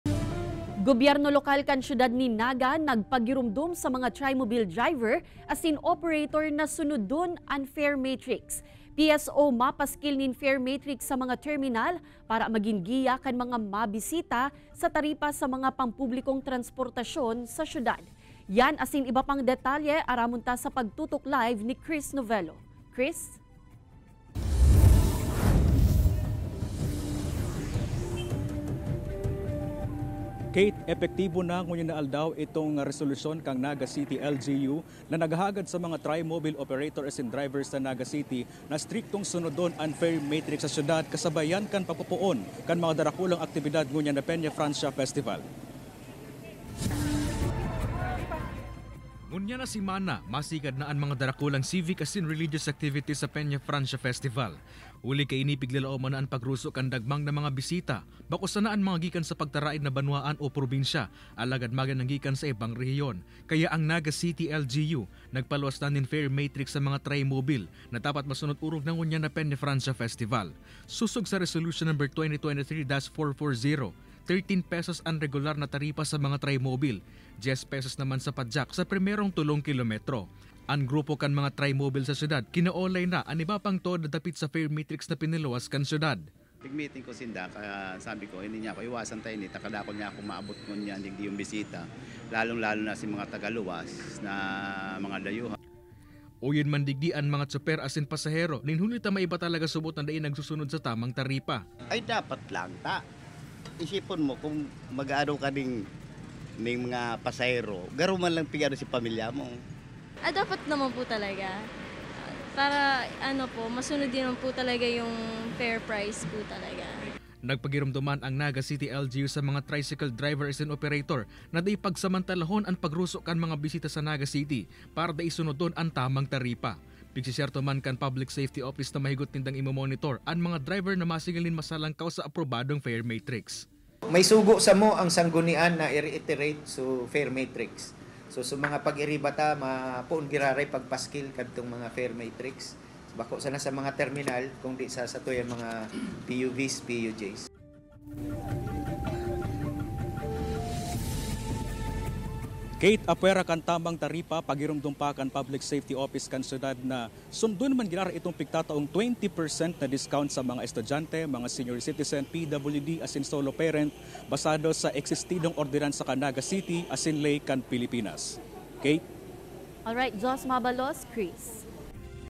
Balitang Bicolandia: Bawal an pasaway
Naga City LGU, nagpagirumdom sa mga tricycle driver asin operator na magsunod sa fare matrix ngonian na kapiyestahan